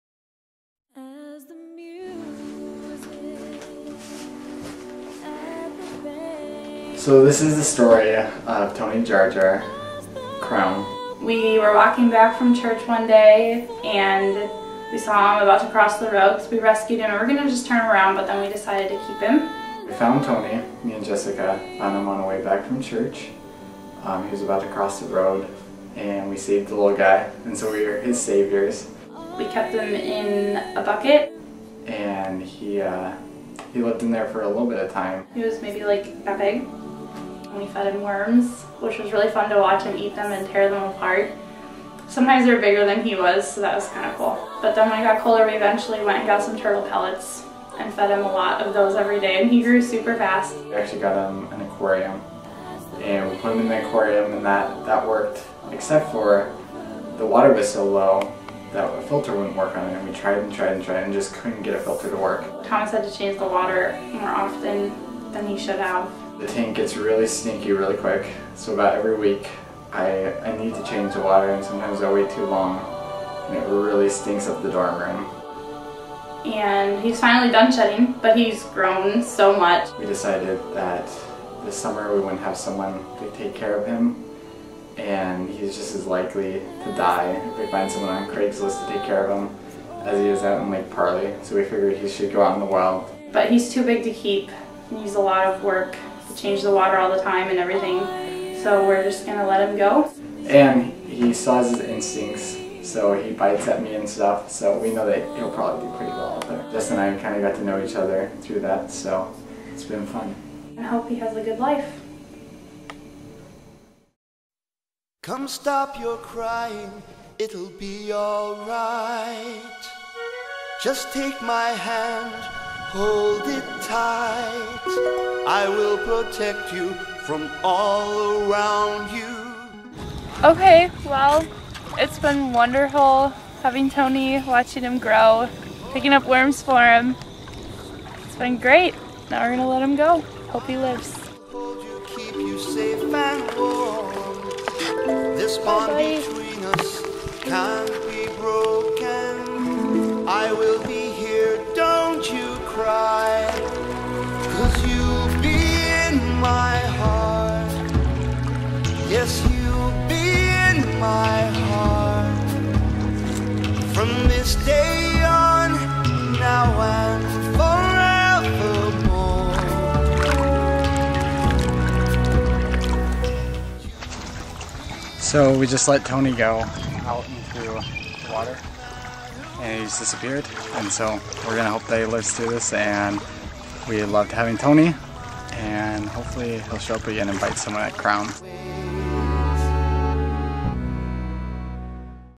Farewell Tony
We once had a turtle. now he is gone